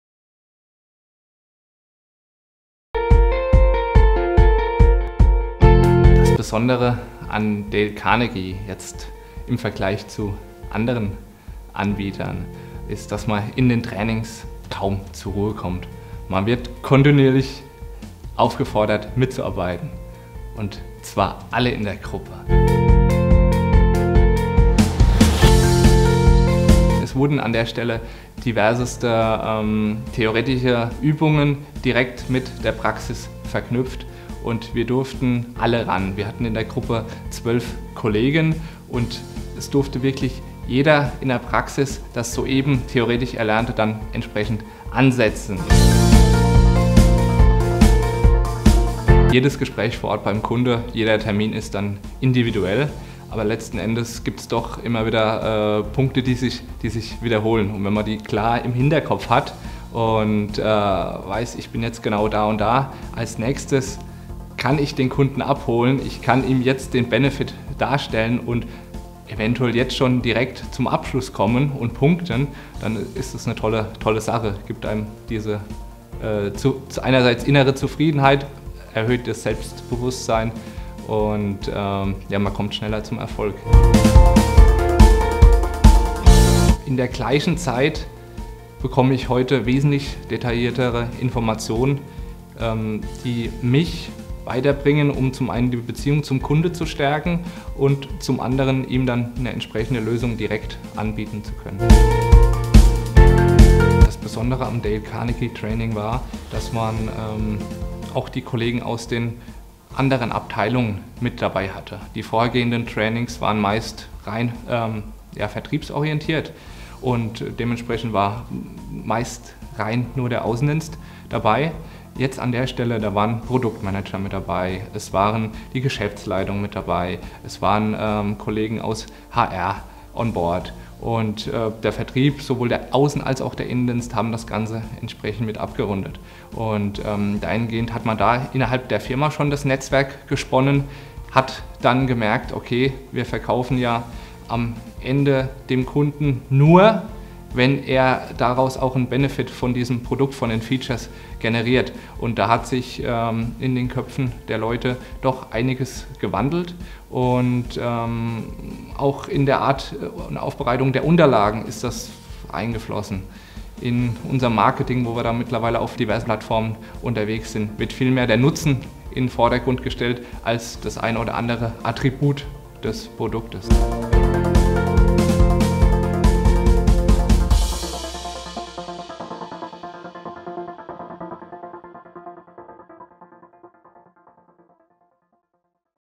Dale Carnegie Testimonial: Lukas Rothermich, field manager, WIKA
Lukas Rothermich speaks from the employee perspective about his experience in the training, the positive changes in his daily work achieved through the more effective sales process and inter-divisional cooperation for value selling.